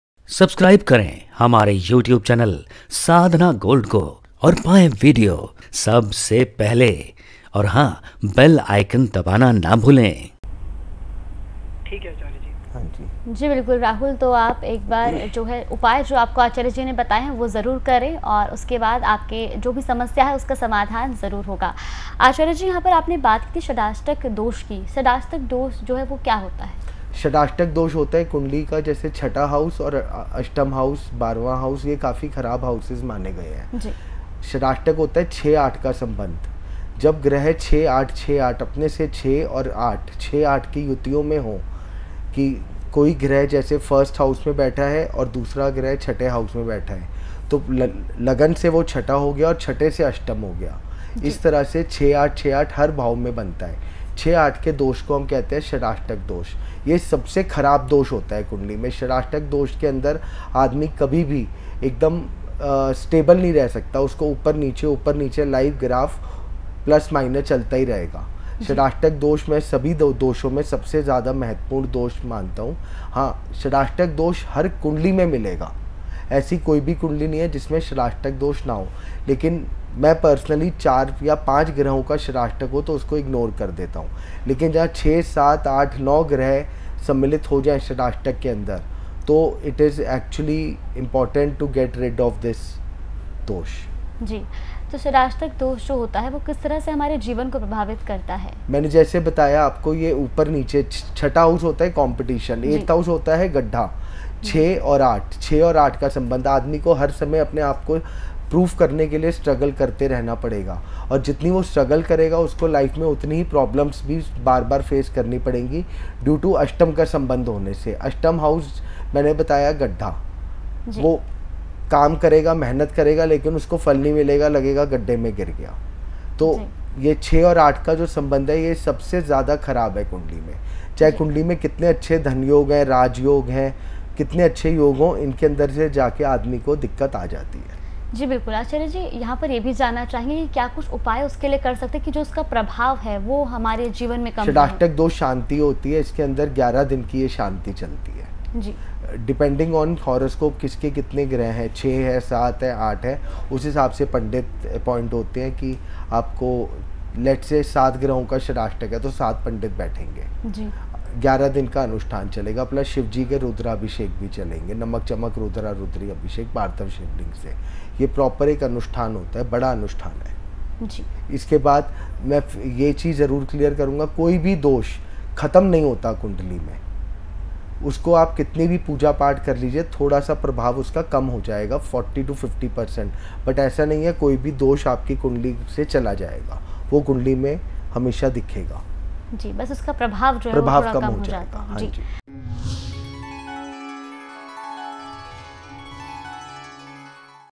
षडाष्टक दोष क्या है, इसका प्रभाव व् उपाय ? ज्योतिषाचार्य मनीष अग्रवाल जी। Sadhna TV
Thanks.